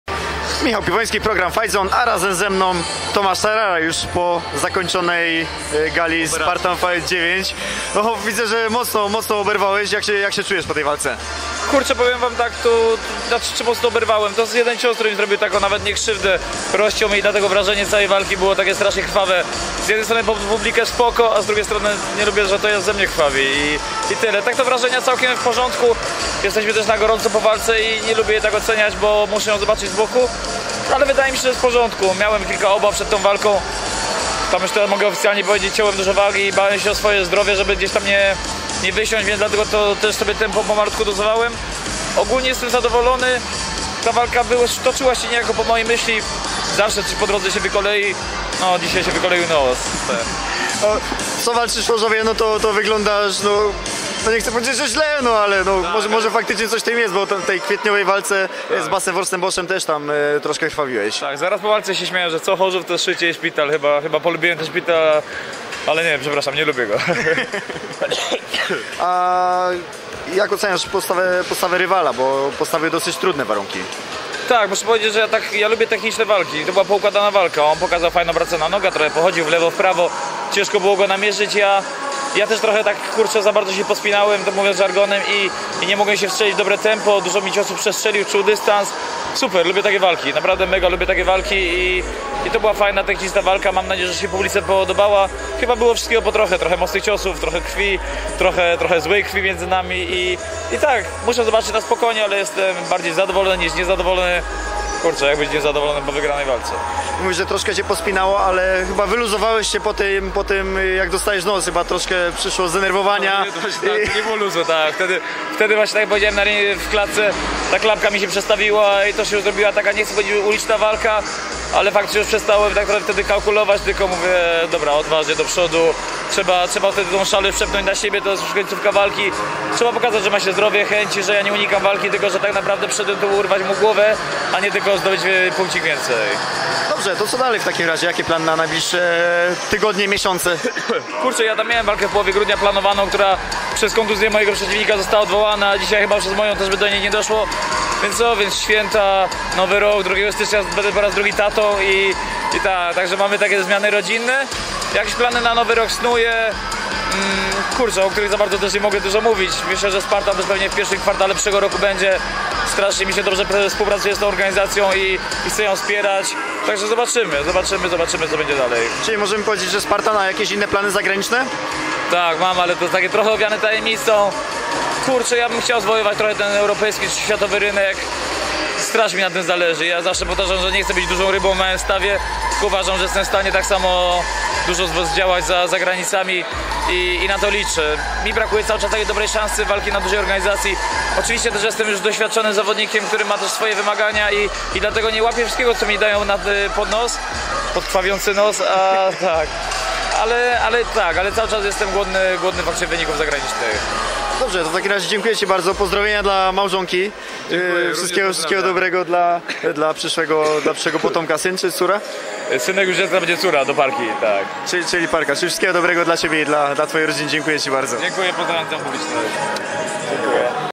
Tomasz Sarara: "Nie chcę być dużą rybą w małym stawie"
Tomasz Sarara w walce z Arturem Gorlovem mocno krwawił, ale ostatecznie to on okazał się zwycięzcą. "Co Chorzów, to szycie i szpital" - mówił po walce Sarara.